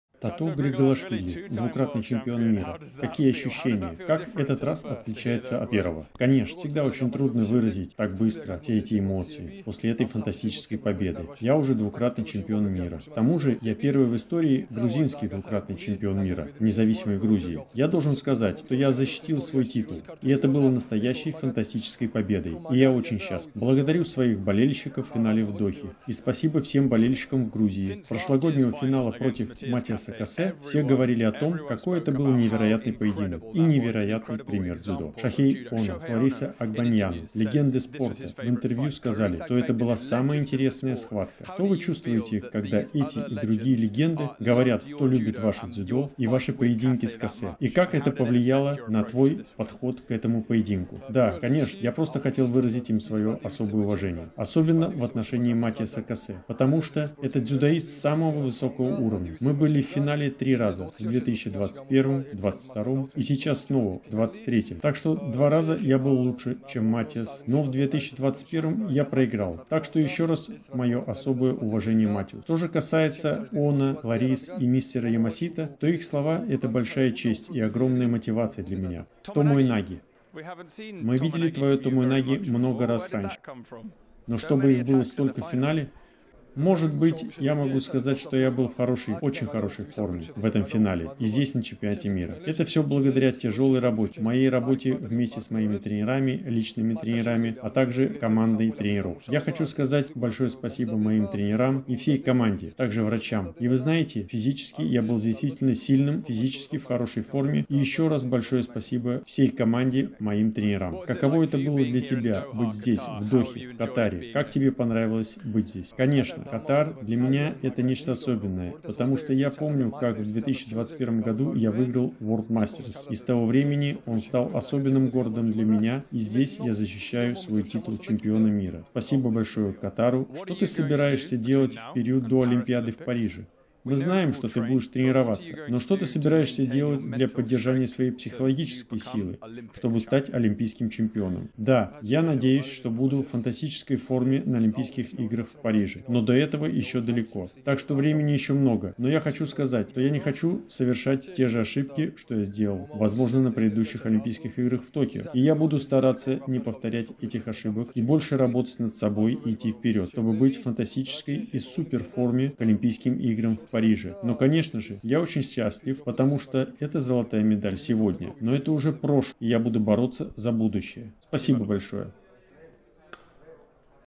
Тато ГРИГАЛАШВИЛИ (Грузия) - Дзюдо Чемпионат Мира Доха 2023 ЗОЛОТОЙ Meдалист/- 81 кг
Тато Григалашвили, чемпион мира по дзюдо 2023 в Катаре, Доха, в весовой категории до 81 кг.
Интервью после награждения.
Озвучено на русском языке Романом Карасевым.